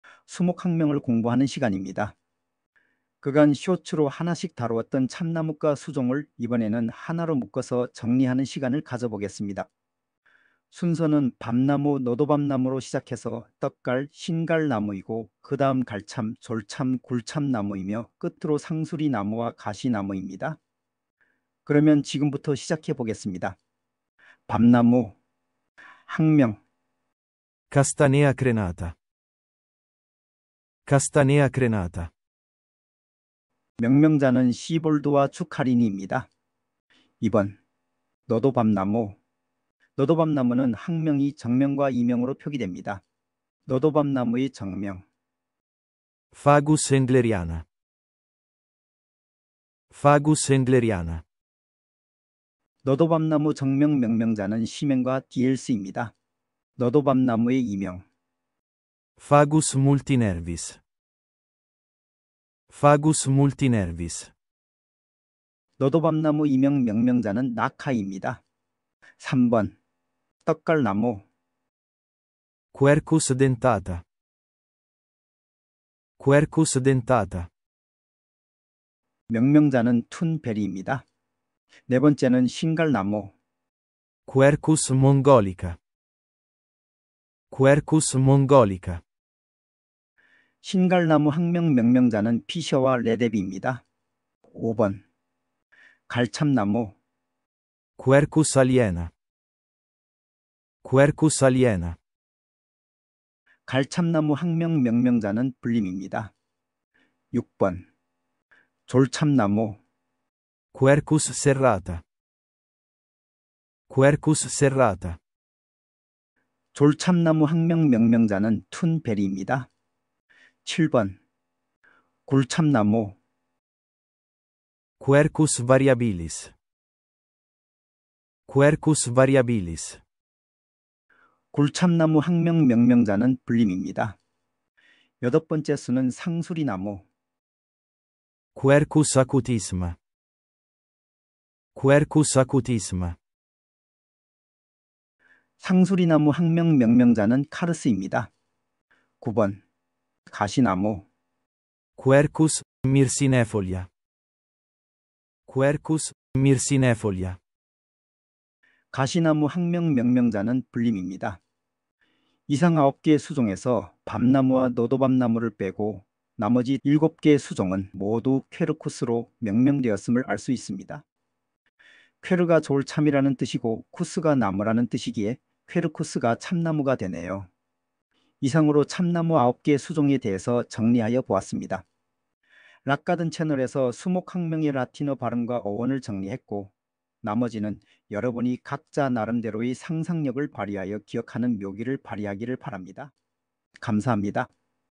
[수목학명] 참나무 리뷰편
기존 숏츠로 만든 9개 참나무 수종을 한장에 집계하여 리뷰하는 영상입니다. 숏츠와 리뷰편을 통으로 보기를 원하면 "참나무 묶음편"을 보시면 됩니다...